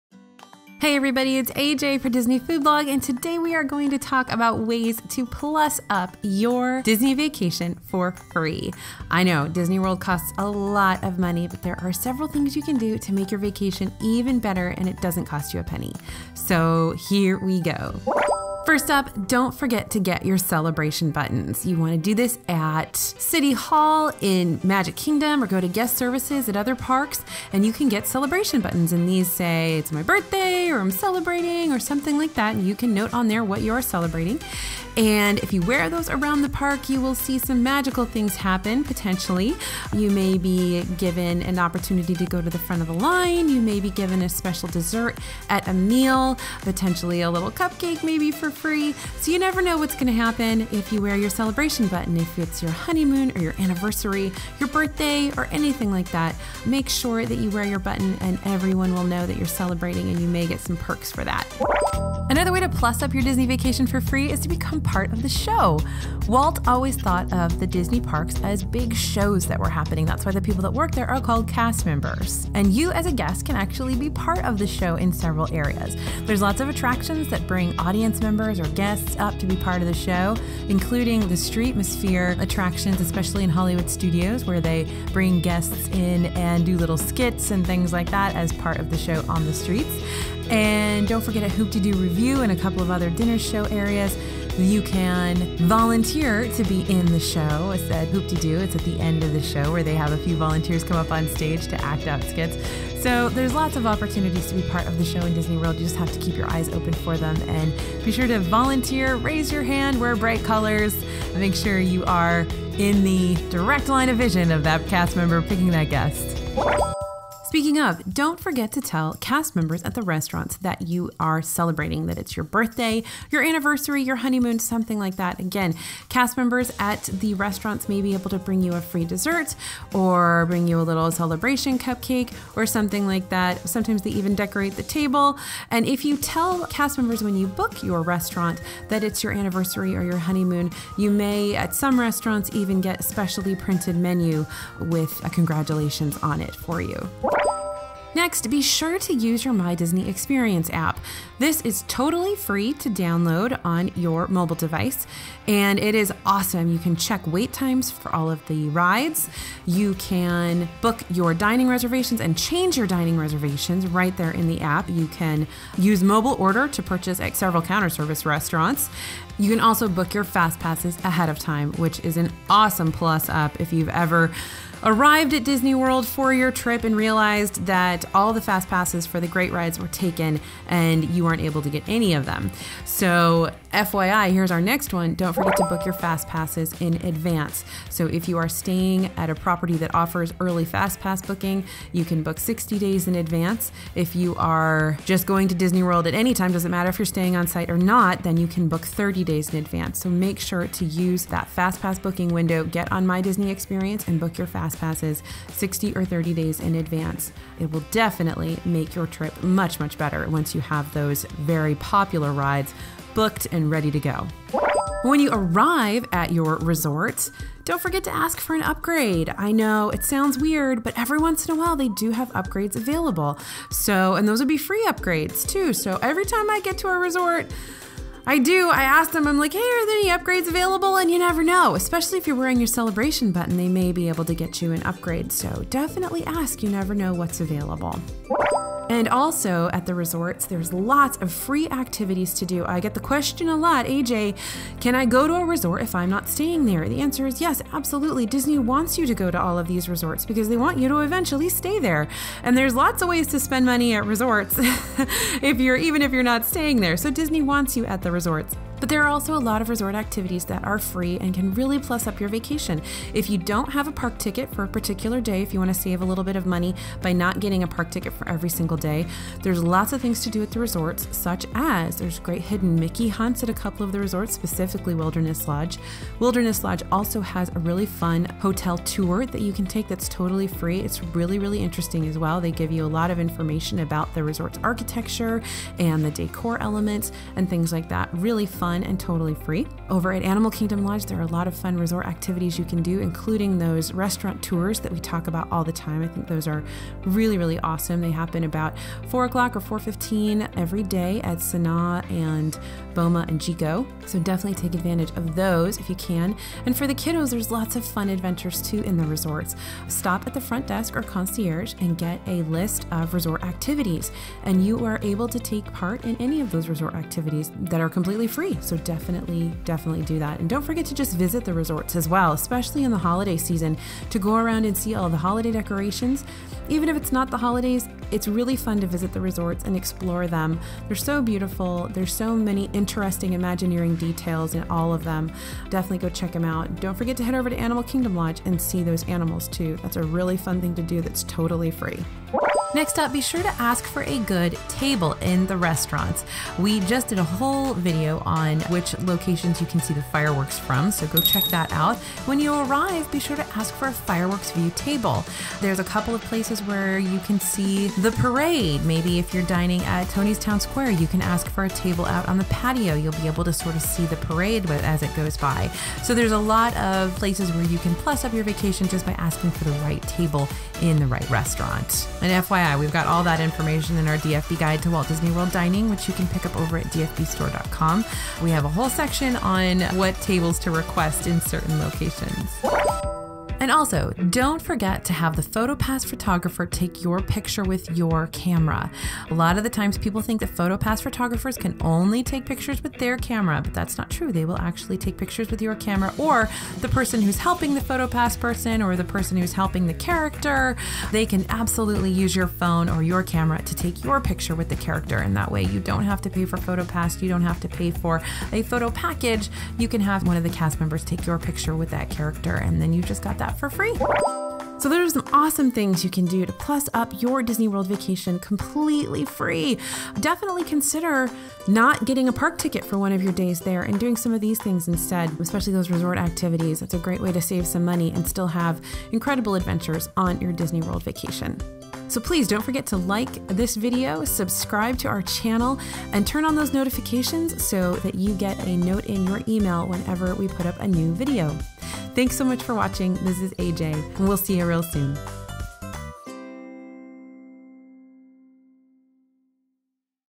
9 FREE Ways To Upgrade Your Disney Vacation
Did you know you can upgrade your Disney vacation FOR FREE? In this video we let you in on our little secrets. Have you tried any of these?